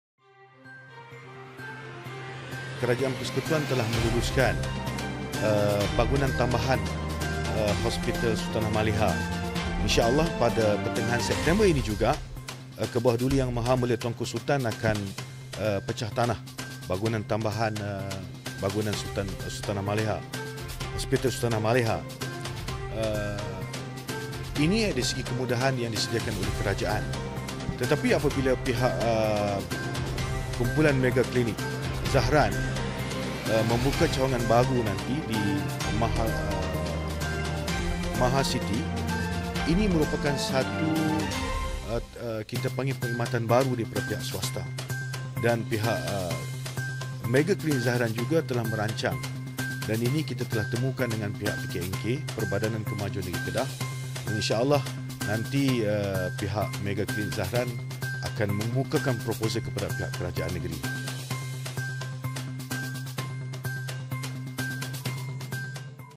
Kelengkapan fasiliti kesihatan di Langkawi tarik pelancong
Kelengkapan fasiliti kesihatan kerajaan mahupun swasta merancakkan lagi sektor pelancongan kesihatan di Langkawi.

Pegawai Daerah Langkawi, Datuk Abdul Gafar Yahya berkata, selaras itu, pelbagai projek dan aktiviti bakal dilaksana bagi menarik pelancong kesihatan domestik mahupun antarabangsa.

Jelas beliau, Kerajaan Persekutuan juga telah meluluskan bangunan tambahan Hospital Sultanah Maliha.

Beliau ditemui selepas Pelancaran Langkawi International Folding Bike Challenge di Kuala Lumpur.